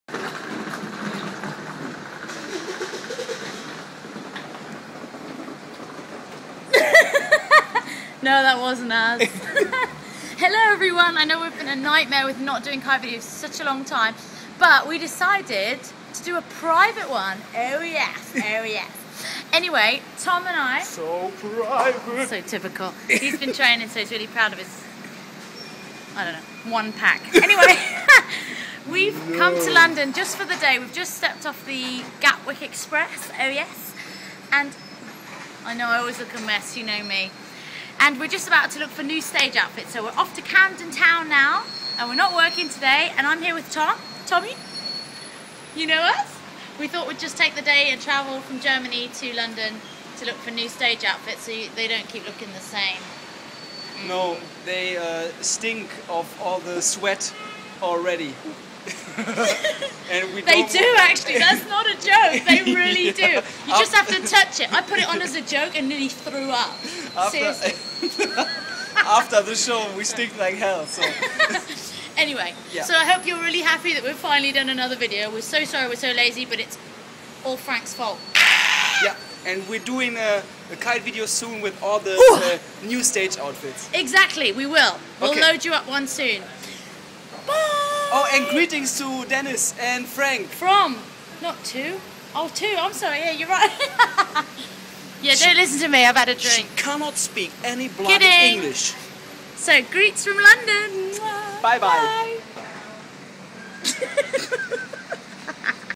Nat in London
Natalie is shopping in London